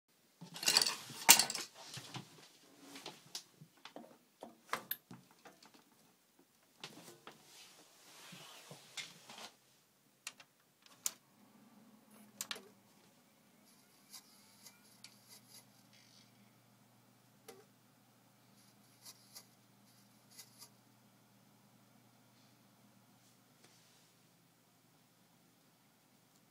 What's wrong with my computer
A while back my computer ( dell dimension 3000 ) stopped working it made a strange noise then smelt of burning. Now every time I try to turn it on gets stuck in a short of standby mode and the turns off. It sounds like the fan shorts out or somethin.  I would Apreciate your help if anyone here on YouTube knows what is wrong with my computer.